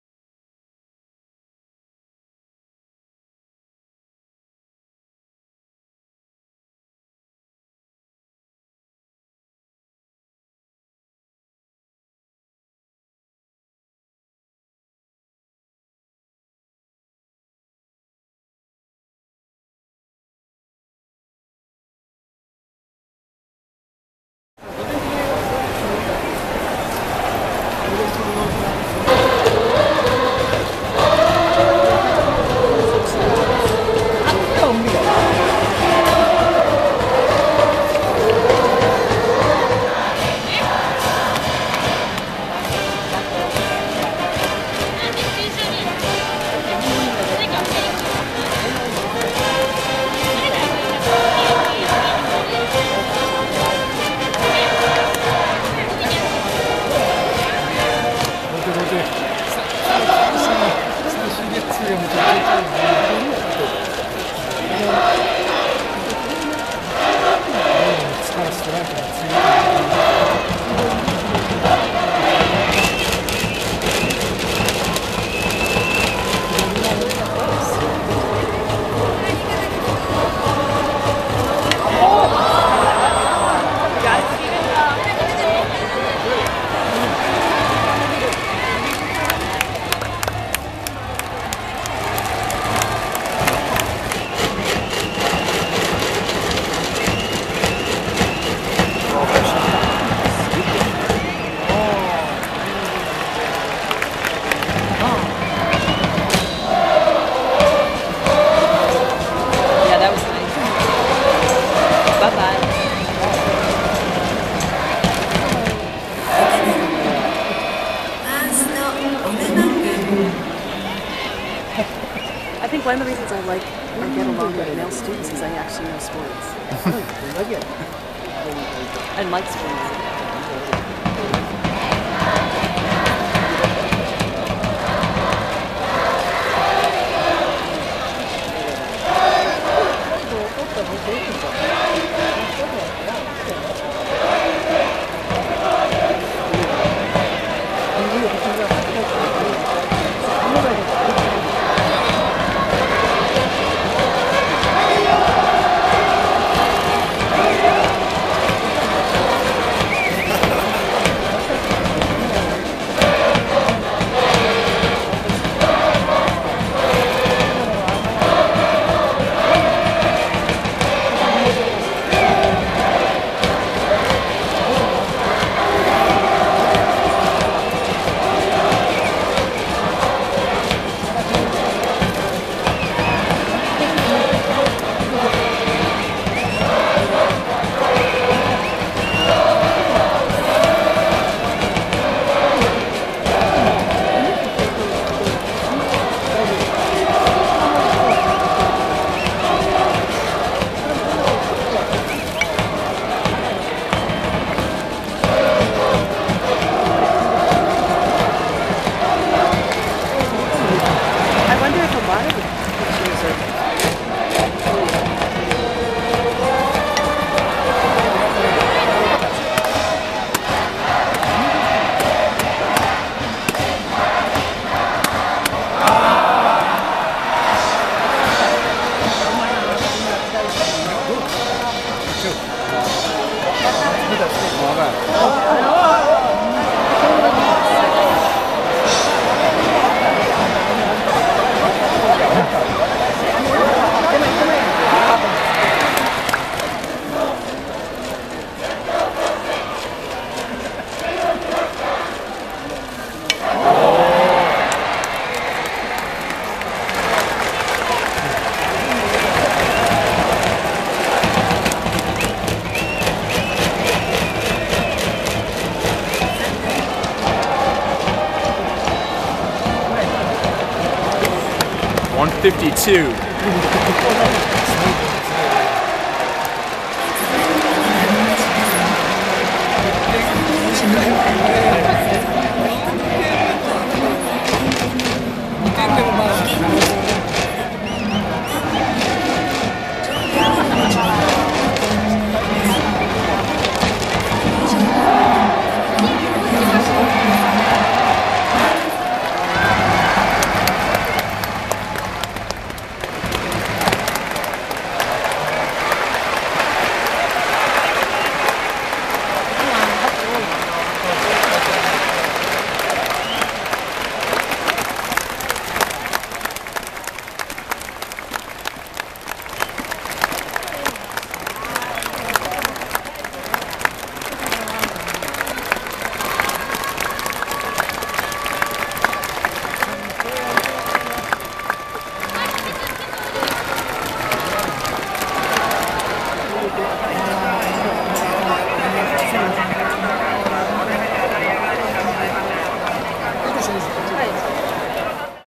Anraku: 94 MPH on 183rd Pitch | １５２KPHー安楽ー済美の怪物２年生
94 MPH at 4:27 on his 183rd pitch. Tomohiro Anraku, the 16-year-old phenom out of Ehime, Japan, was made famous after hurling 772 pitches in 5 games over 9 days in the Spring Koshien High School Baseball Tournament.

In the Summer Koshien High School Baseball Tournament, Anraku threw 183 pitches in his final game during the second round. Here is some video highlighting his pitching repertoire and rhythm during his last game. His final pitch was clocked at 152 KPH, or 94 MPH. (4:27 marker in the video)

It should be noted that his velocity increased as he went deeper into the game, which started at 8 AM. His fastball sat in the upper eighties, but he was able to rev it up to 92 when needed, and even hit 94 a few times. Similarly, his control improved later in the game, as he was able to effectively bury his hard slider in on lefties.

Additionally, Anraku, batting cleanup, hit a three-run home run in the bottom of the 10th inning to bring his team within one run. Unfortunately, his heroic efforts fell short, and his team lost 7-6 to Hanamaki Higashi, out of Iwate Prefecture.